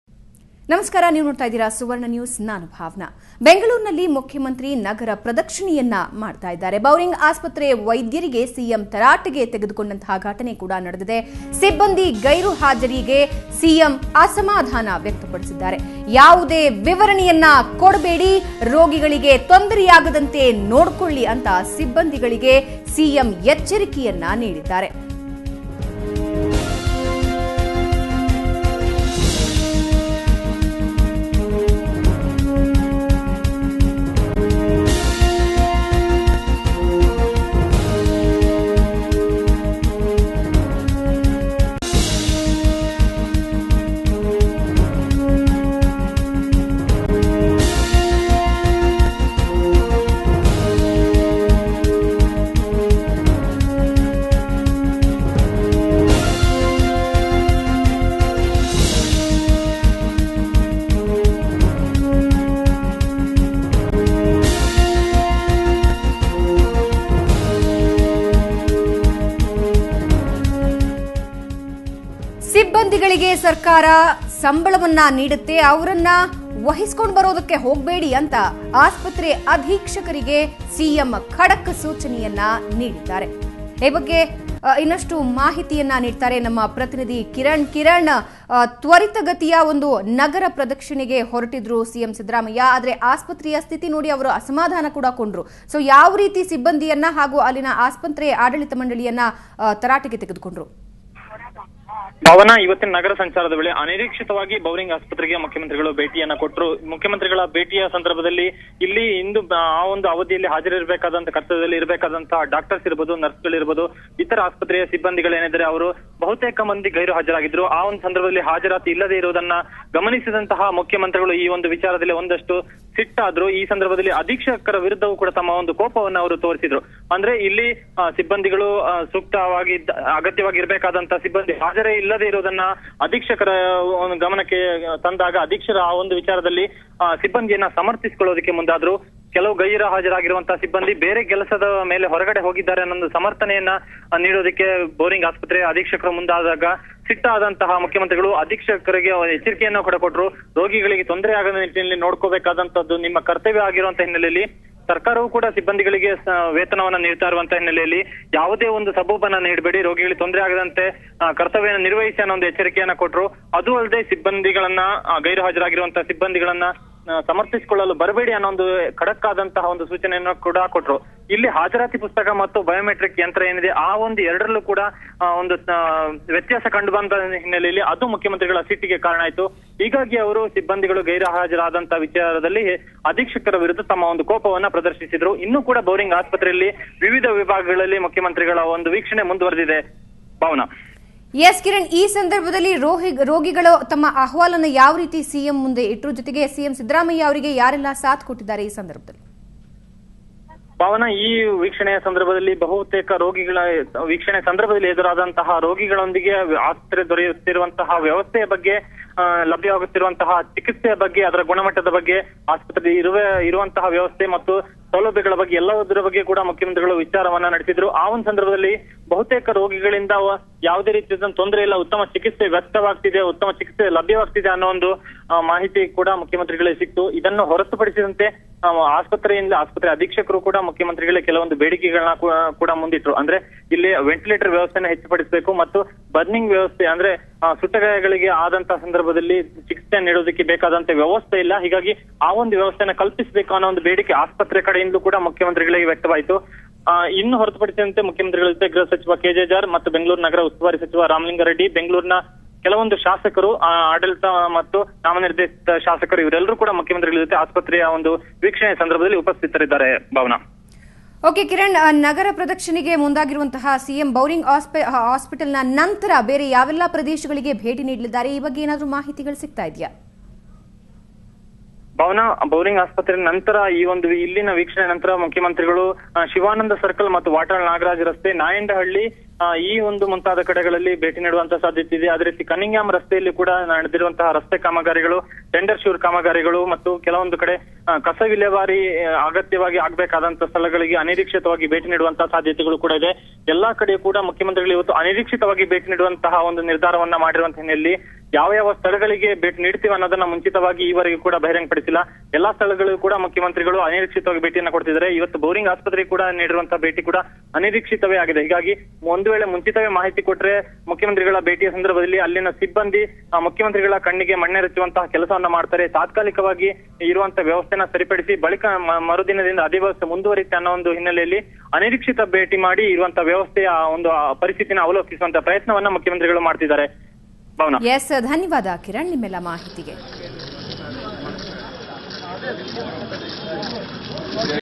Siddaramaiah found the pathetic condition of Bangalore after his city rounds
Suvarna News 24X7 - 15 May 15 , Siddaramaiah found the pathetic condition of Bangalore after his city rounds